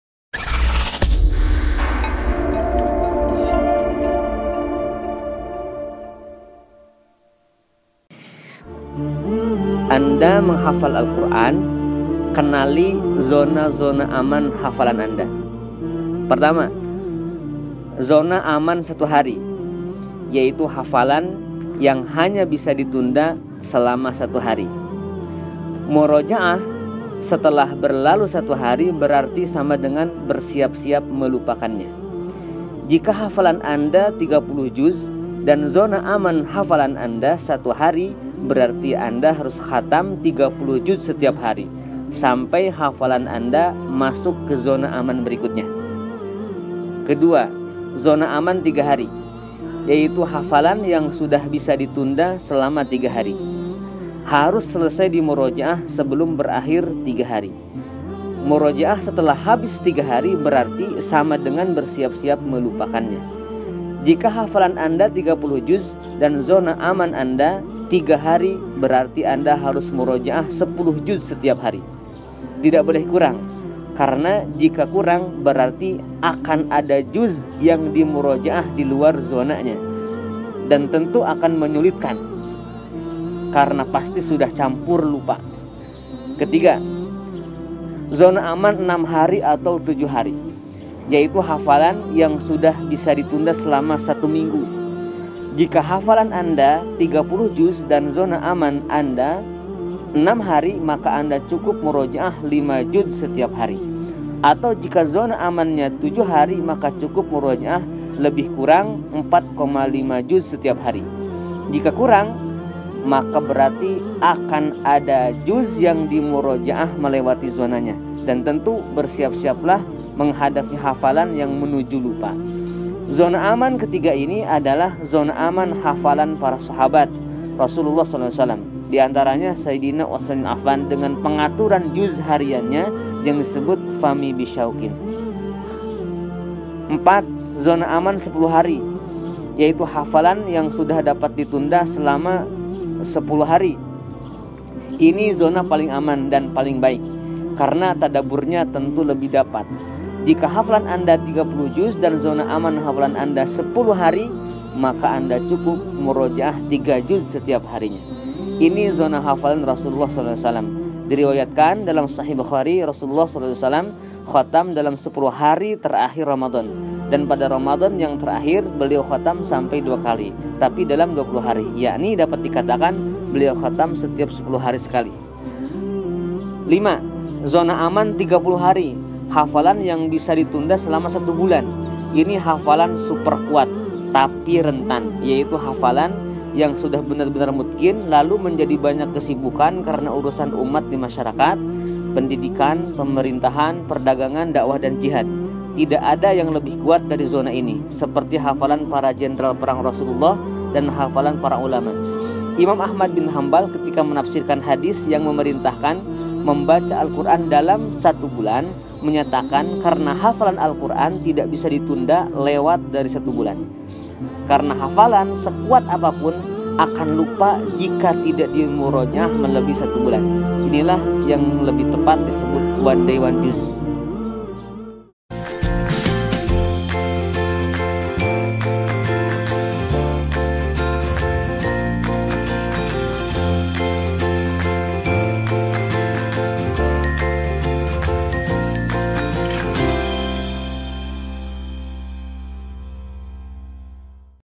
ANDA MENGHAFAL AL-QURAN, KENALI ZONA-ZONA AMAN HAFALAN ANDA
1. Zona Aman 1 Hari. Hafalan yang hanya bisa ditunda 1 hari. Murojaah setelah berlalu 1 hari berarti sama dengan bersiap-siap melupakannya. Jika hafalan Anda 30 juz dan zona aman hafalan Anda 1 hari berarti Anda harus khatam 30 juz setiap hari sampai hafalan Anda masuk ke zona aman berikutnya.

2. Zona Aman 3 Hari. Hafalan yang sudah bisa ditunda selama tiga hari. Harus selesai dimurojaah sebelum berakhir 3 hari. Murojaah setelah habis 3 hari berarti sama dengan bersiap-bersiap melupakannya. Jika hafalan Anda 30 juz dan zona aman Anda 3 hari berarti Anda harus murojaah 10 juz setiap hari. Tidak boleh kurang. Karena jika kurang berarti akan ada juz yang dimurojaah di luar zonanya. Dan tentu akan menyulitkan. Karena pasti sudah campur lupa.

3. Zona Aman 6 Hari atau 7 Hari. Hafalan yang sudah bisa ditunda selama 1 minggu. Jika hafalan Anda 30 juz dan zona aman Anda 6 hari maka Anda cukup murojaah 5 juz setiap hari. Atau jika zona amannya 7 hari maka cukup murojaah lebih kurang 4,5 juz setiap hari. Jika kurang, maka berarti akan ada juz yang dimurojaah melewati zonanya. Dan tentu bersiap-siaplah menghadapi hafalan yang menuju lupa. Zona aman ketiga ini adalah zona aman hafalan para sahabat Rasulullah Saw. Di antaranya sayyidina Utsman bin Affan r.a. dengan pengaturan juz hariannya yang disebut fami bisyaiqin (فمي بشوق).

4. Zona Aman 10 Hari. Hafalan yang sudah dapat ditunda selama 10 hari. Ini zona paling aman. Dan paling baik. Karena tadabburnya tentu lebih dapat. Jika hafalan Anda 30 juz dan zona aman hafalan Anda 10 hari maka Anda cukup murojaah 3 juz setiap harinya. Ini zona hafalan Rasulullah Saw. Di riwayatkan dalam Shahih al-Bukhari, Rasulullah Saw khatam dalam sepuluh hari terakhir Ramadhan. Dan pada Ramadhan yang terakhir, beliau khatam sampai 2 kali, tapi dalam 20 hari. Yakni dapat dikatakan, beliau khatam setiap sepuluh hari sekali.

5. Zona Aman 30 Hari. Hafalan yang bisa ditunda selama 1 bulan. Ini hafalan super kuat. Tapi rentan. Yaitu hafalan yang sudah benar-benar mutqin lalu menjadi banyak kesibukan karena urusan umat di masyarakat, pendidikan, pemerintahan, perdagangan, dakwah, dan jihad. Tidak ada yang lebih kuat dari zona ini. Seperti hafalan para jenderal perang Rasulullah Saw dan hafalan para ulama. Imam Ahmad bin Hanbal ketika menafsirkan hadits yang memerintahkan membaca Al-Quran dalam 1 bulan menyatakan karena hafalan Al-Quran tidak bisa ditunda lewat dari 1 bulan. Karena hafalan, sekuat apapun, akan lupa jika tidak dimurojaah melebihi 1 bulan. Inilah yang labih tepat disebut one day one juz.

Deden Muhammad Makhyaruddin
Indonesia Murojaah Foundation